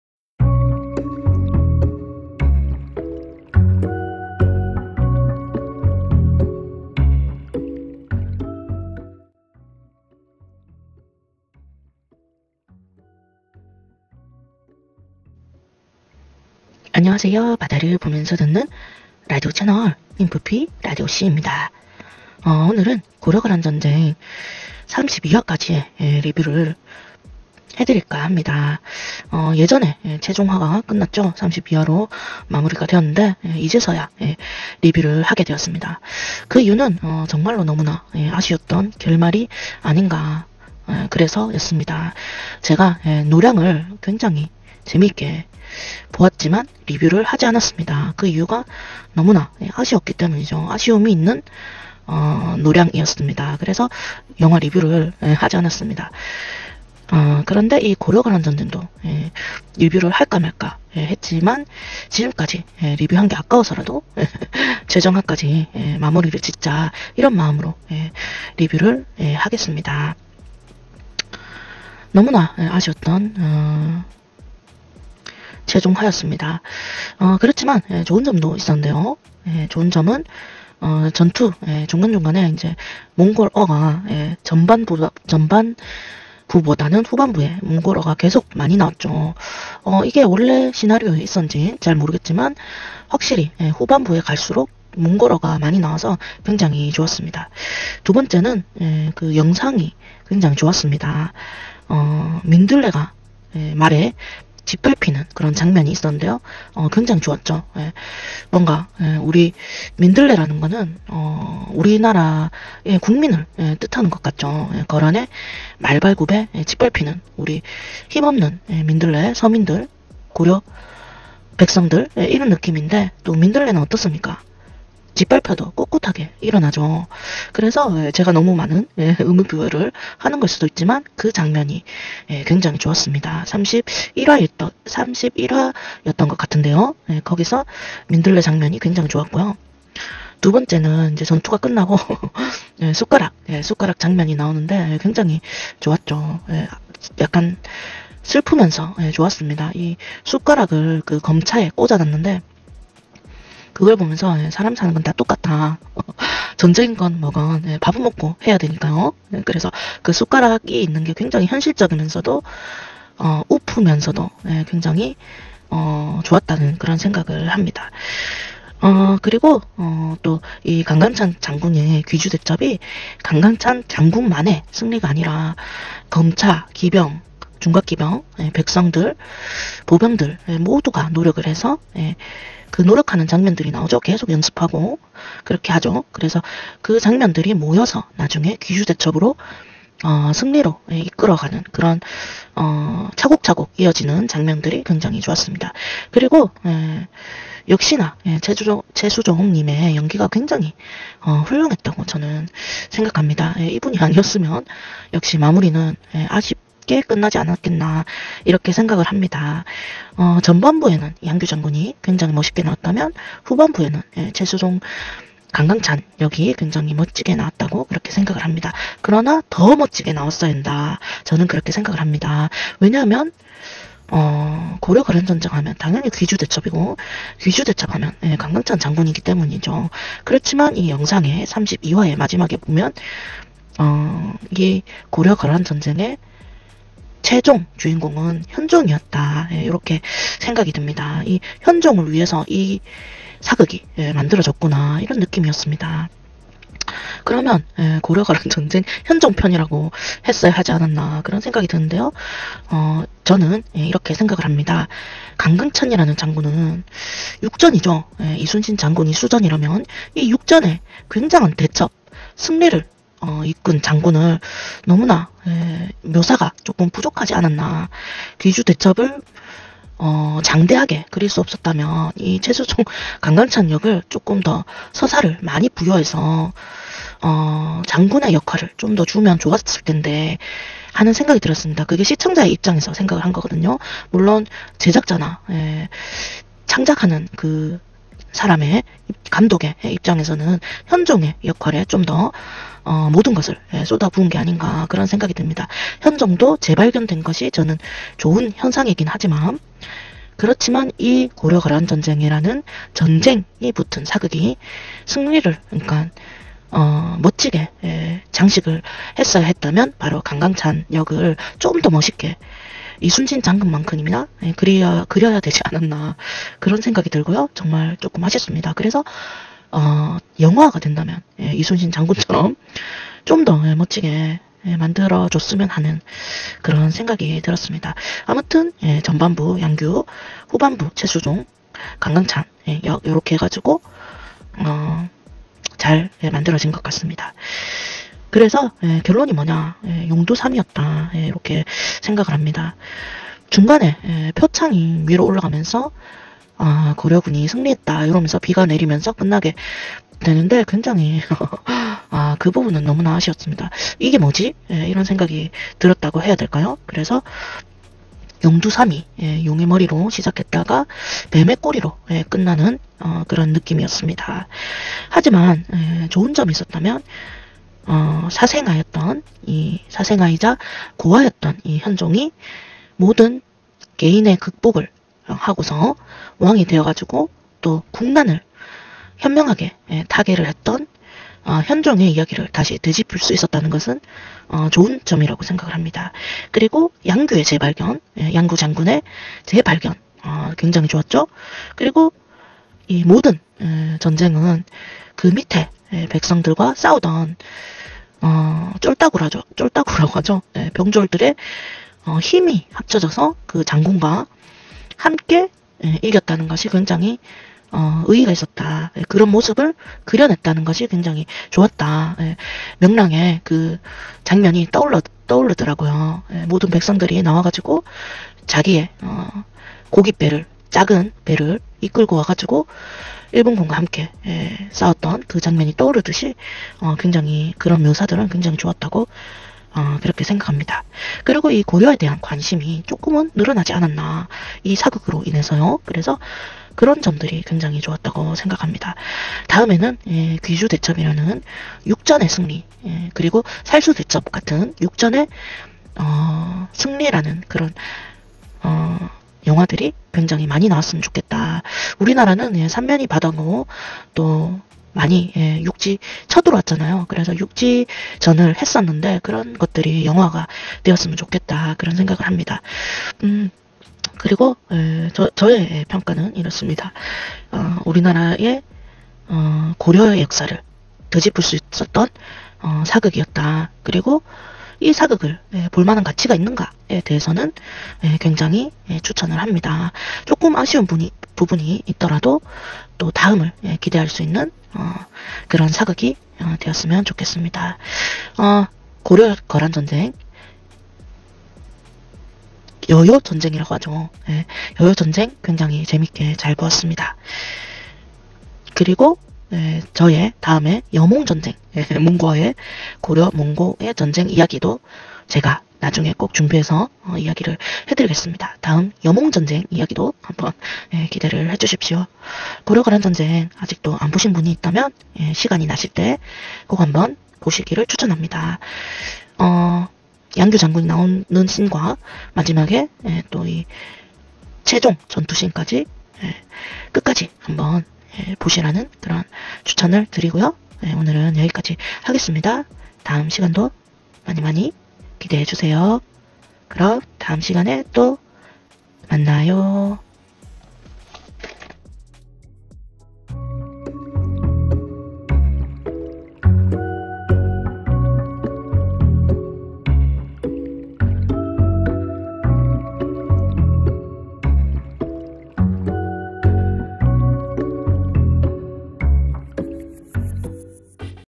고려거란전쟁 23~32화 후기와 최종 총평
KBS 고려거란전쟁 드라마 후기 | 인프피라디오씨 132회
오늘은 [고려거란전쟁 드라마 후기 최종 완결편]입니다.